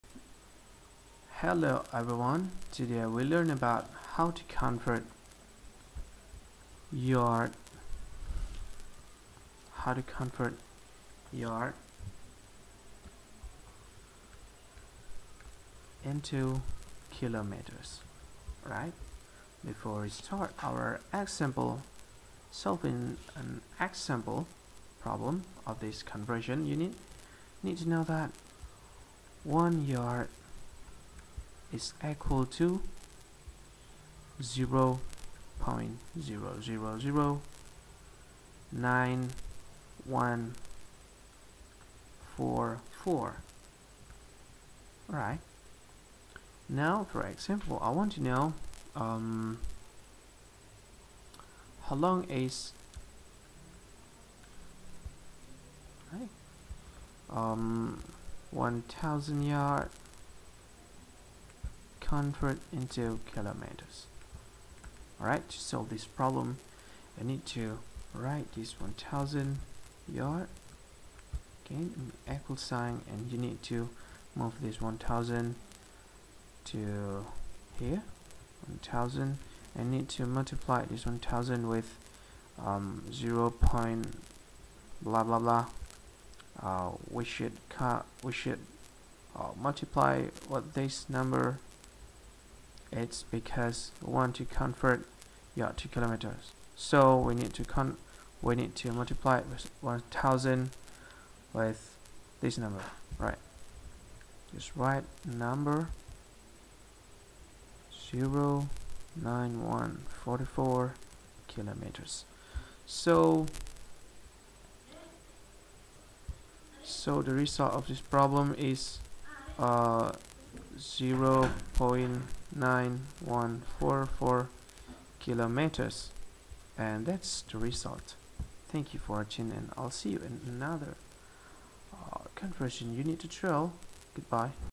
Convert Yard to Kilometer (Yard to Km) - Example and Formula
How to Convert Yard to Kilometer (Yard to Km) ?
this tutorial will show you how to Convert Yard to Kilometer (Yard to Km) with example and formula